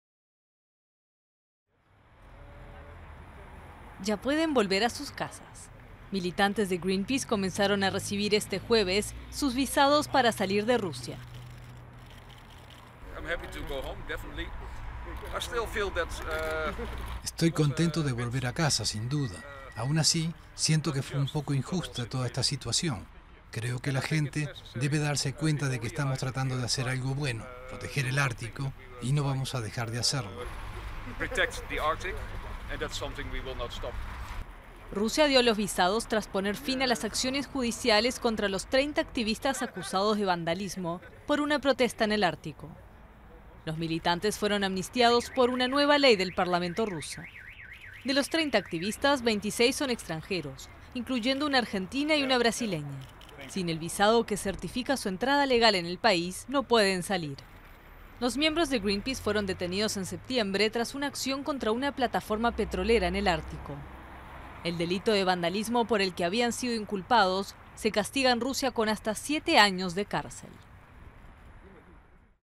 Militantes de Greenpeace reciben visados para salir de Rusia
Rusia comenzó este jueves a entregar visados a los militantes de Greenpeace amnistiados, tras haber puesto fin a las acciones judiciales contra ellos.